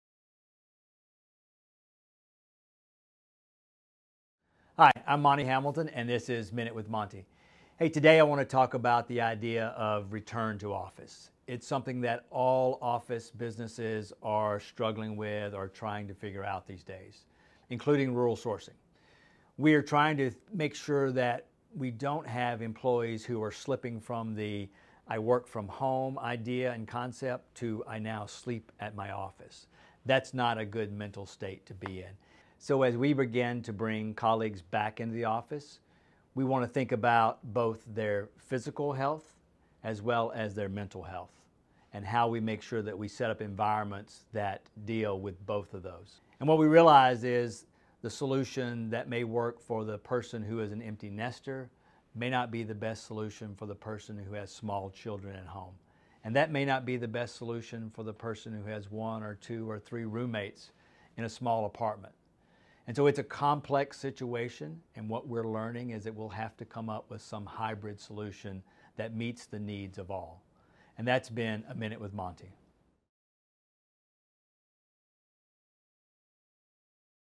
MinuteWithMonty: Returning to the Office
When should we return to the office? It’s a question leaders are grappling with. Some companies have already started going back while others are hesitant. In this week’s MinuteWithMonty, Monty Hamilton dives into the pitfalls of working from home and what you should consider before going back to the office.